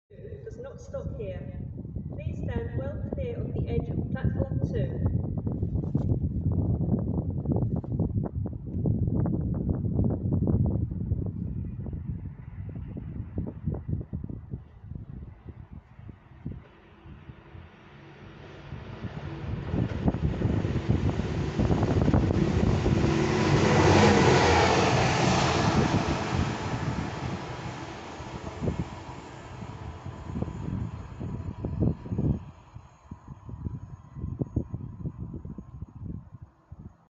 **Bright Lights** Blinding Track machine at Market Harborough 19/10/22
I would like to give a big shout out to sue for making this possible. I went down to Market Harborough to see this track machine. Sadly I got not one or wave but it was cool to see.